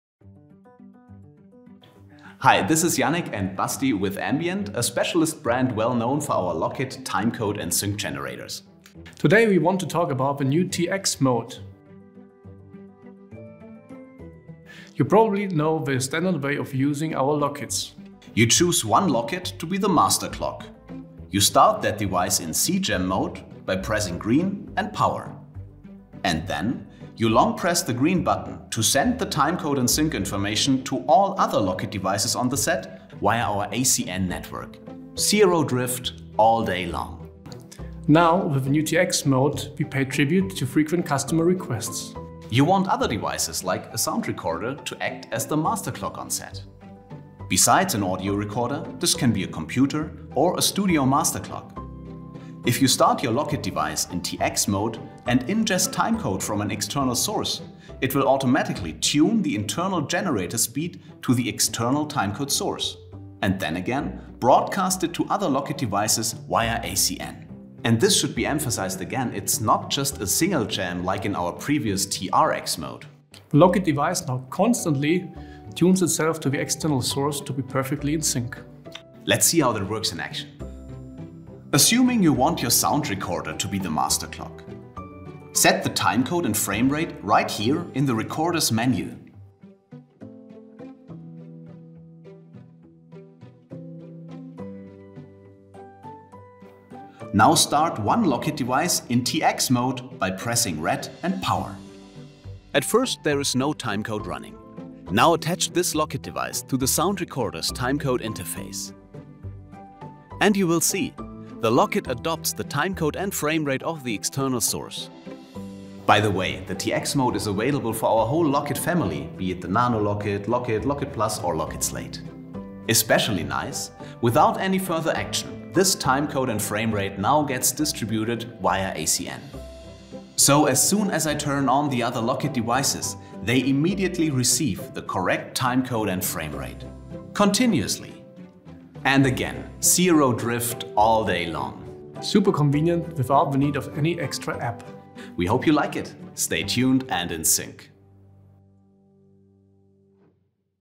Lockit Timecode - Firmware 8 | TX Mode explained
Dear Lockit Timecode users,

with the new Firmware 8 we introduce the improved TX Mode. Using this mode you can bridge the Timecode from an external device through one Lockit to all other Lockits on the set.

It works for all current members of the Lockit Family (NanoLockit, Lockit, LockitSlate, Lockit+)

- Choose an external device (e.g. Audio Recorder) to be the Master Clock on set
- Start one Lockit devices as TX Master (Hold Red, Press Power)
- Connect it to the external device via cable
- The TX Master will automatically adapt its generator speed to the LTC Timecode source
- The TX Master will broadcast Timecode & Framerate through ACN
- Lockit devices in the same ACN channel will automatically follow the TX Master